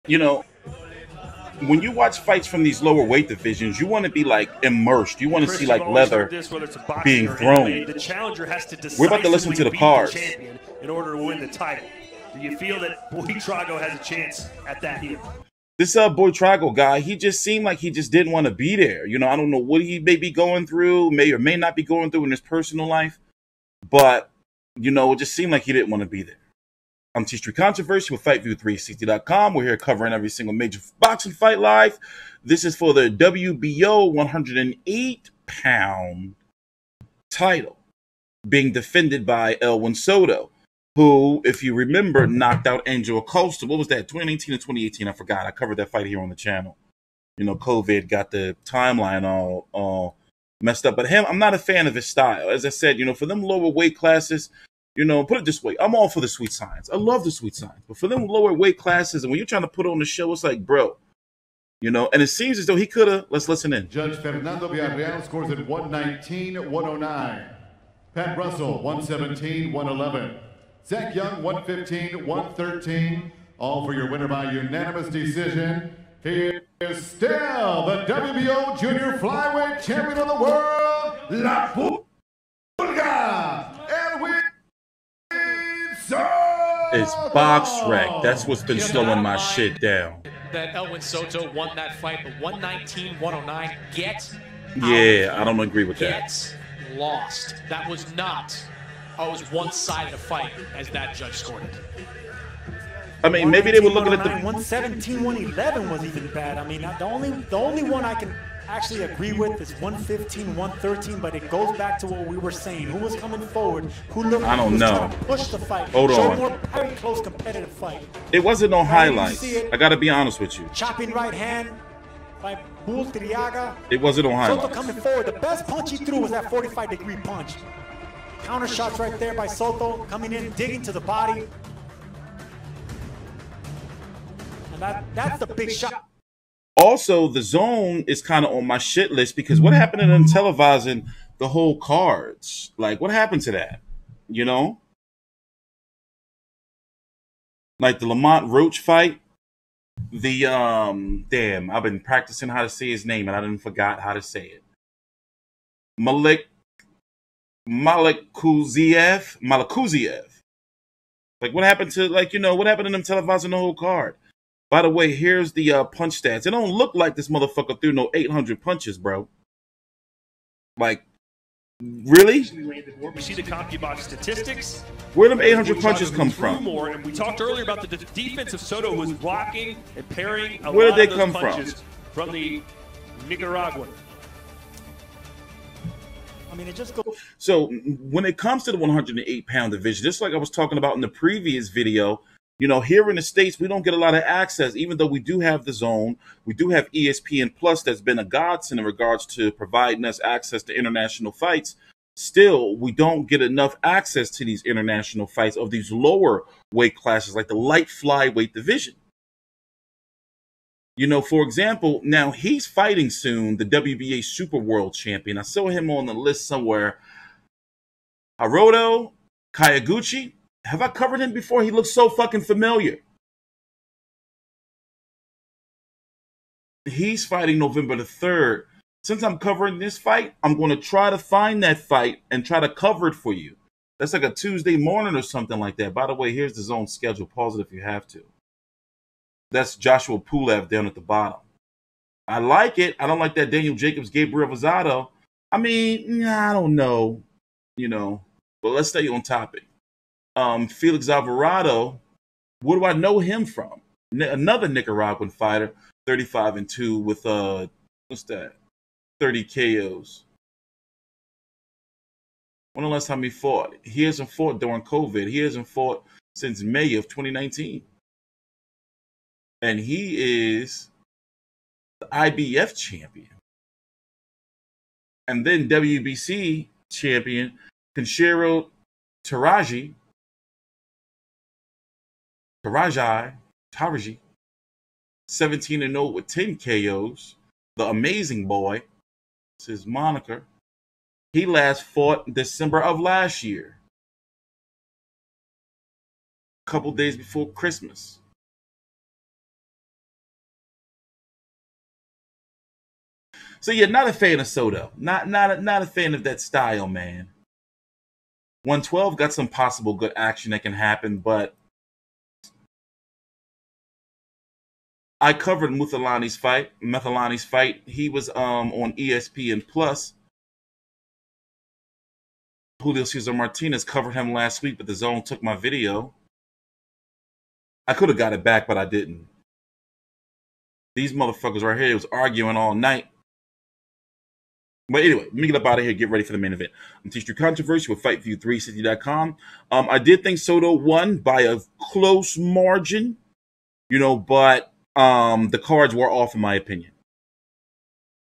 Elwin Soto vs Carlos Buitrago Post Fight Recap & Highlights: BAD Cards - Right Man WON! What's Next?
Also PLEASE Check 👈 for latest boxing news, rankings, schedules, highlights, reviews, & results **